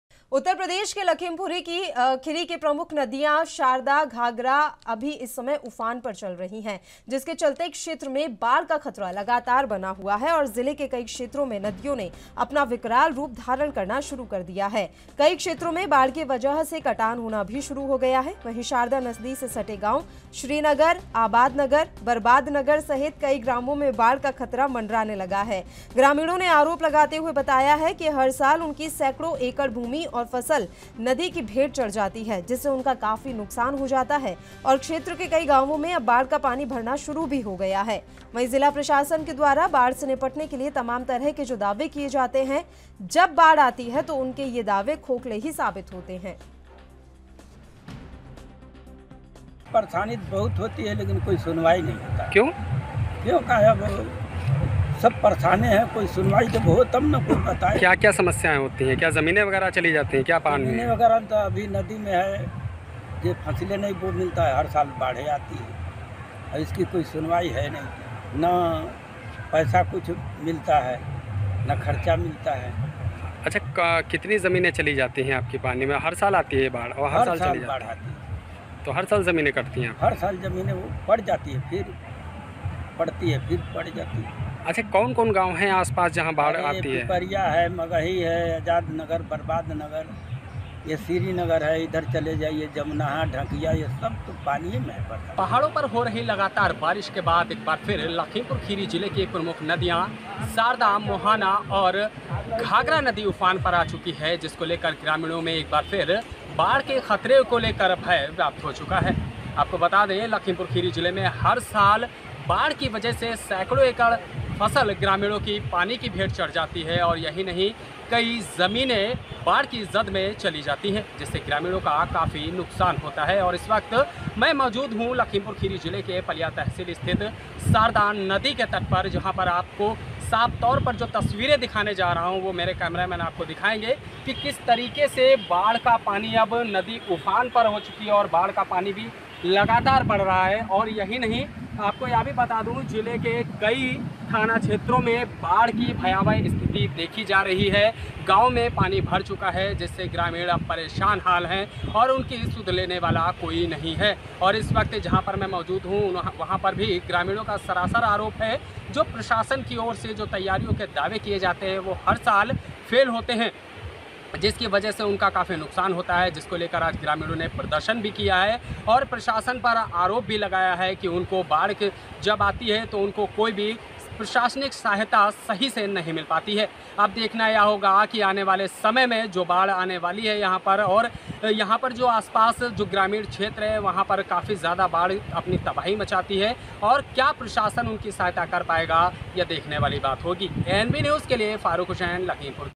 लखीमपुर खीरी की प्रमुख नदियां शारदा,घाघरा उफान पर | ANB NEWS
लखीमपुर खीरी की प्रमुख नदियां शारदा,घाघरा और मोहना नदियां उफान पर आ चुकी है... जिसके चलते अब क्षेत्र में बाढ़ का खतरा लगातार बना हुआ है... और जिले के कई क्षेत्रों में नदियों ने अपना विकराल रूप धारण करना शुरू कर दिया है... जिससे कई क्षेत्रों में बाढ़ की वजह से कटान होना भी शुरू हो गया है... वहीं शारदा नदी से सटे गांव श्रीनगर,आबाद नगर बरबाद नगर सहित कई ग्रामों में बाढ़ का खतरा मंडराने लगा है. ग्रामीणों ने आरोप लगाते हुए बताया है कि... हर साल उनकी सैकड़ों एकड़ भूमि और फसल नदी की भेंट चढ़ जाती है... जिससे उनका काफी नुकसान होता है... और क्षेत्र के कई गांवों में अब बाढ़ का पानी भरना शुरू हो गया है... लेकिन प्रशासन के द्वारा बाढ़ से निपटने के लिए तमाम तरह के जो दावे किए जाते हैं लेकिन जब बाढ़ आती है तो उनके दावे खोखले साबित होते हैं.